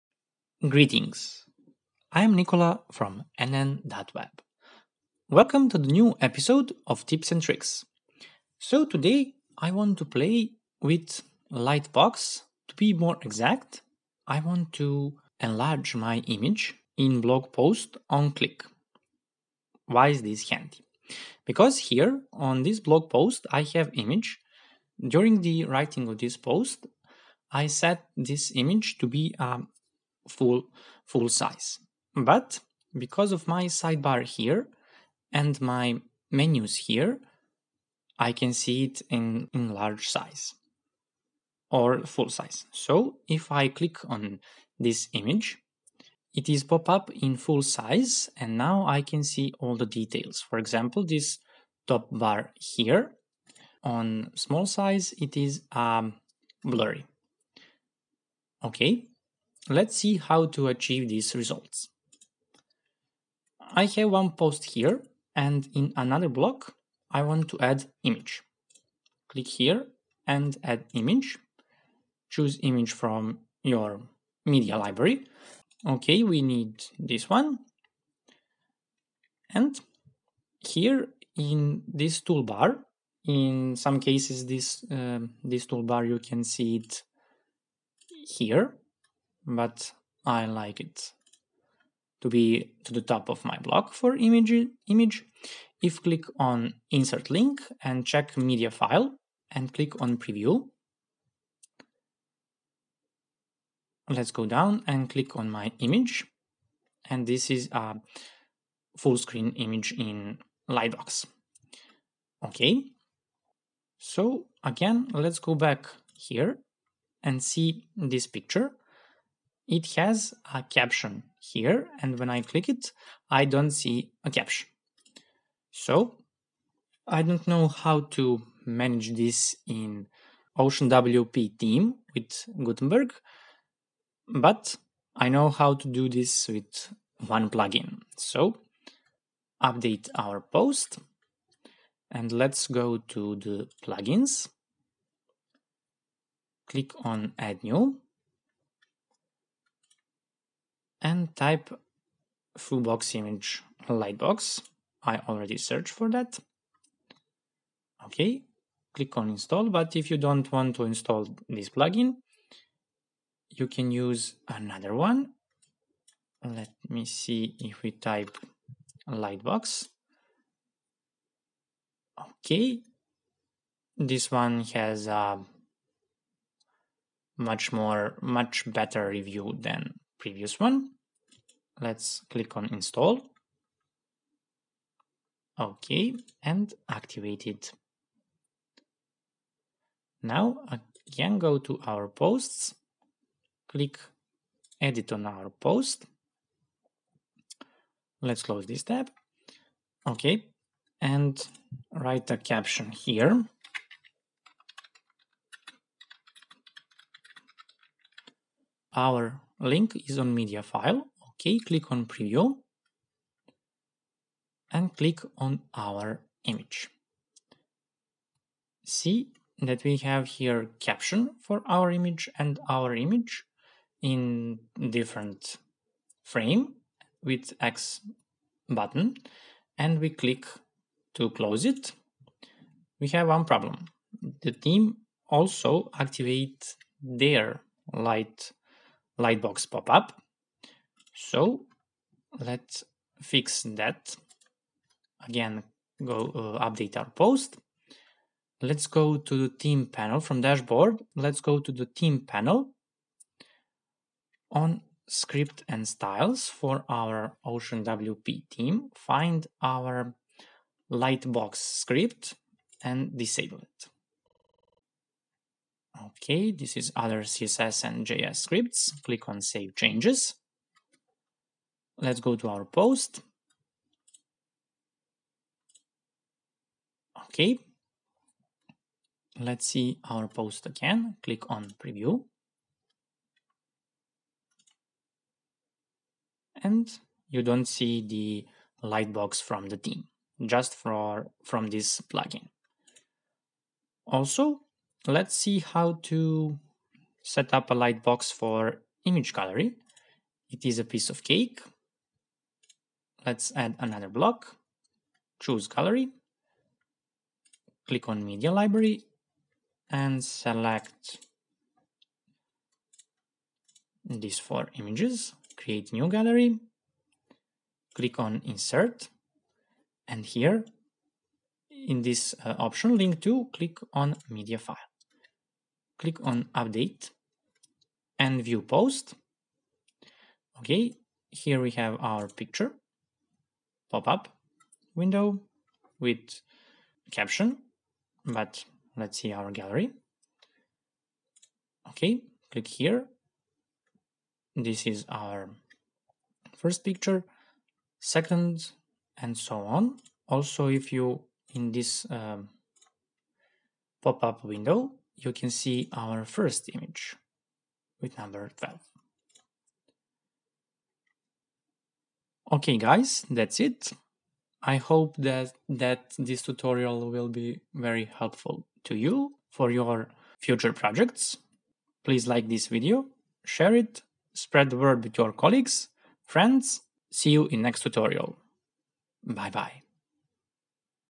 How to Enlarge Photo on click in WordPress
In this video you can find a way to enlarge image on click with the Lightbox. I used "Simple Lightbox" plugin, and disable Lightbox on OceanWP theme and resolve the problem with caption.